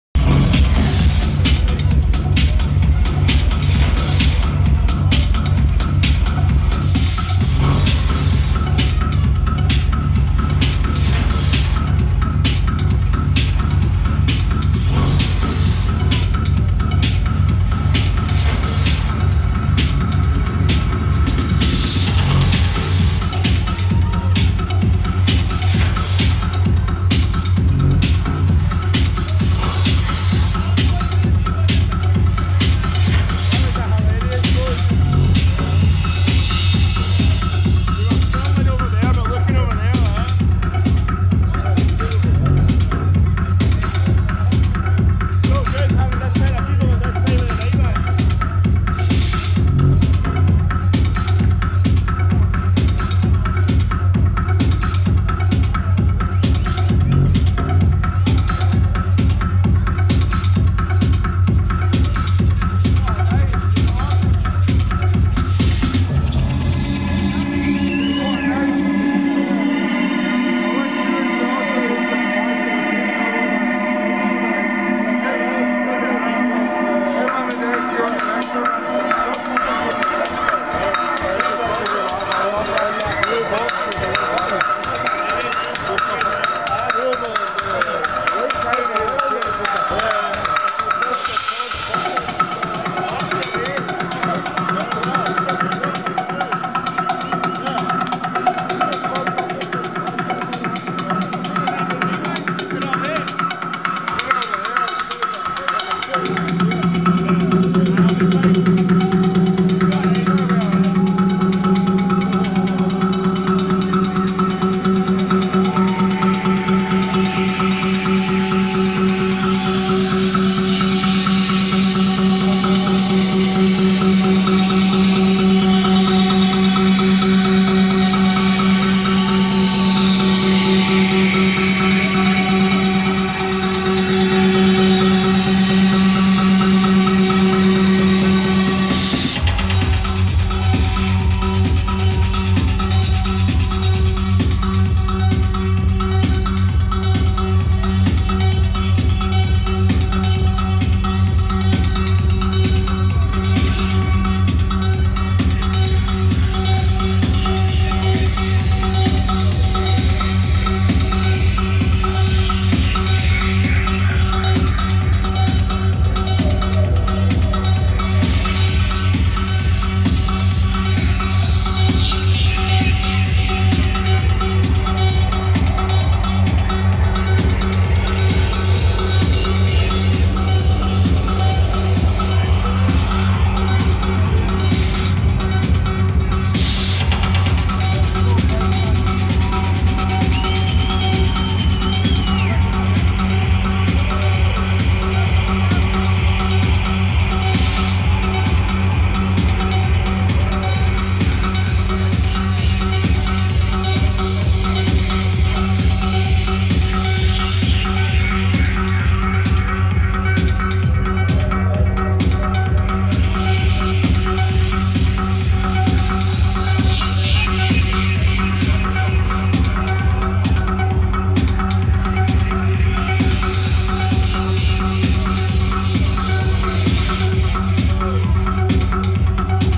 ronin @ Off The Record
ronin from otr ripping it up for the main room